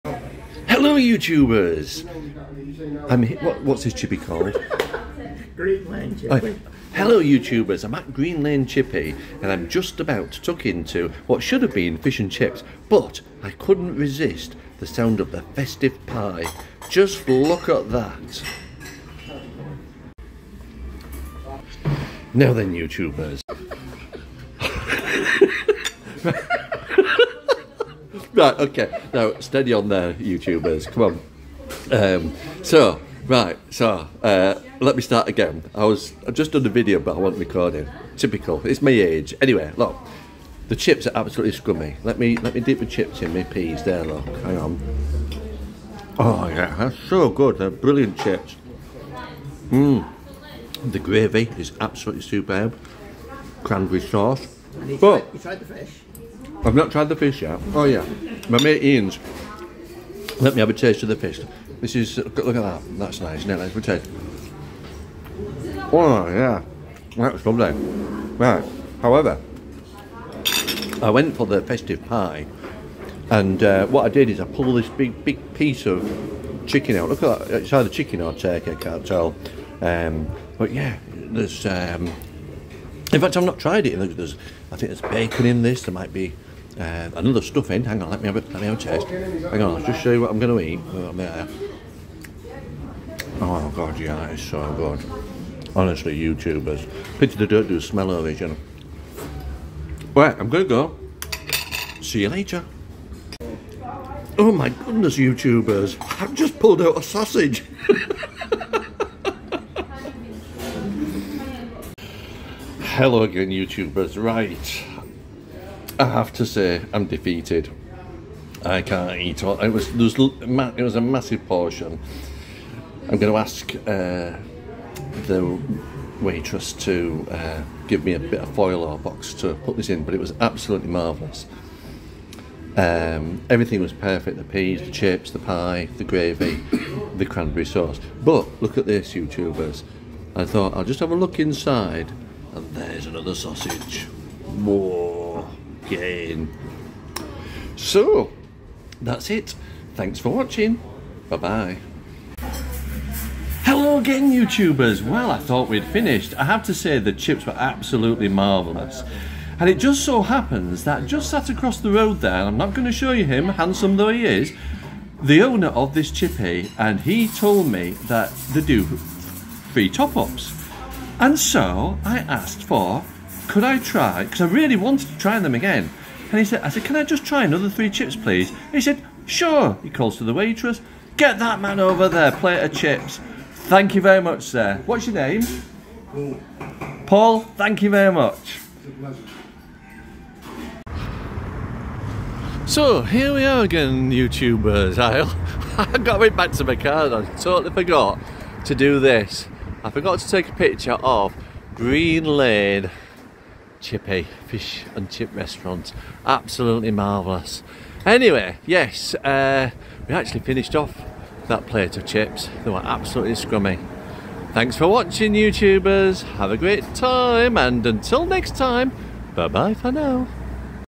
The Best Festive Pie! - Only at Green Lane Chippy (Please Like, Share, and Subscribe - it's free)
Green Lane Chippy, Leigh, near Manchester

Has Wi-Fi

507, 509 Holden Rd, Leigh WN7 2JJ

Many thanks to Paul and the staff